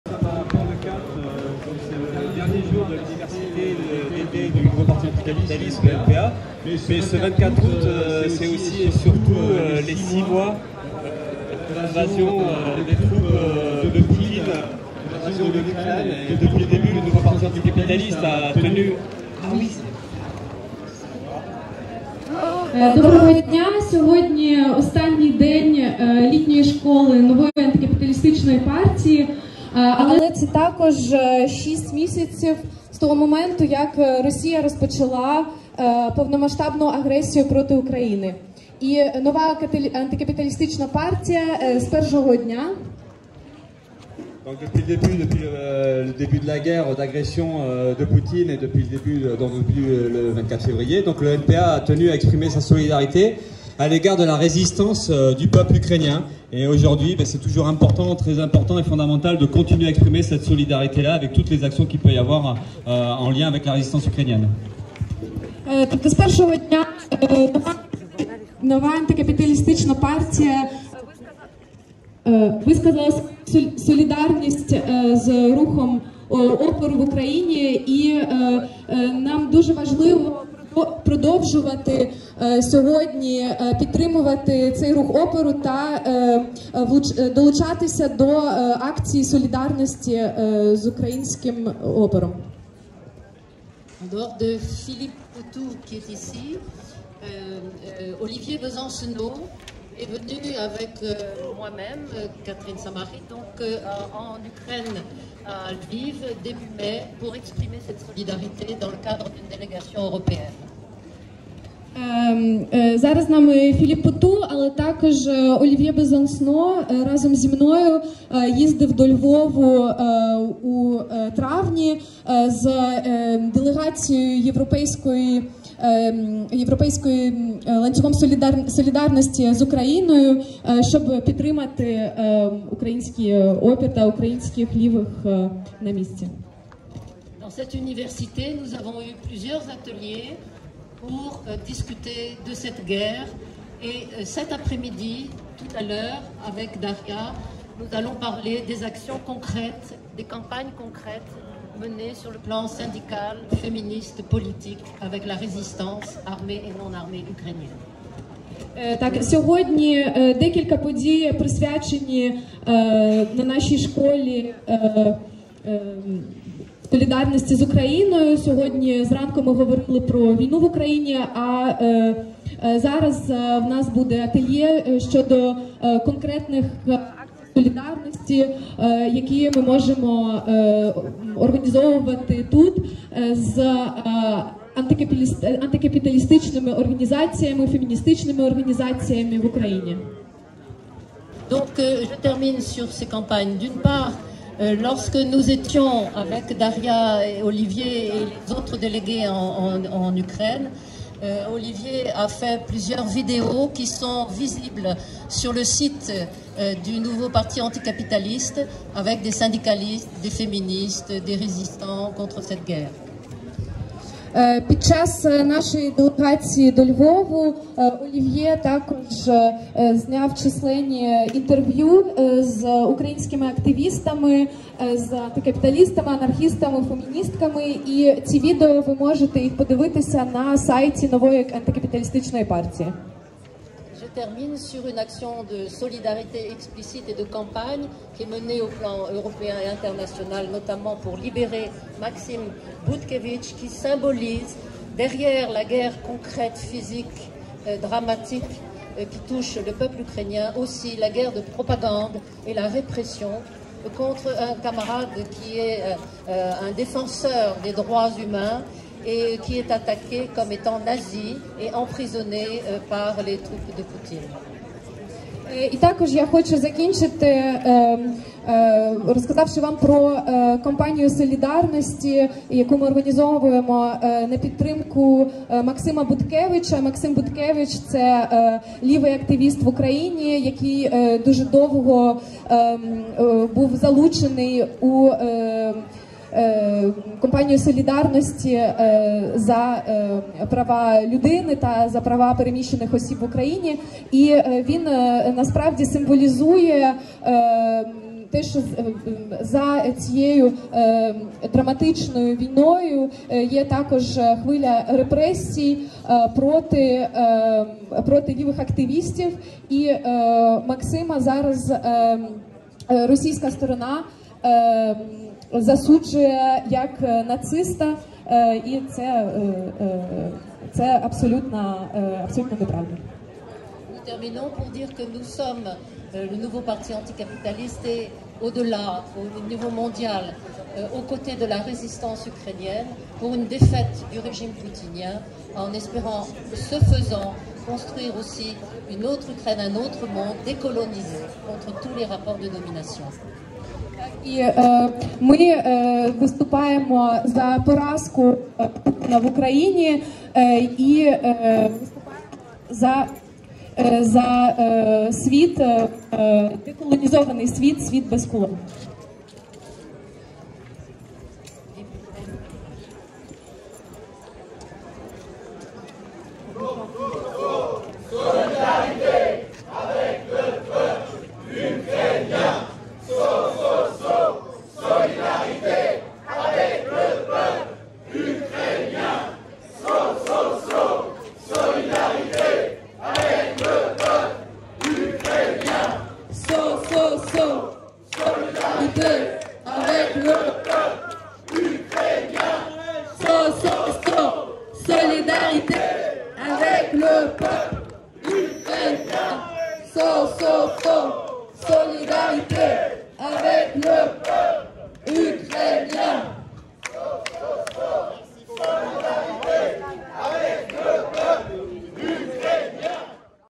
SOUTIEN AU PEUPLE UKRAINIEN (UNIVERSITÉ D'ÉTÉ DU NPA)
Rassemblement de soutien au peuple ukrainien par le NPA.
Avec Philippe Poutou, Catherine Samary et Daria Saburova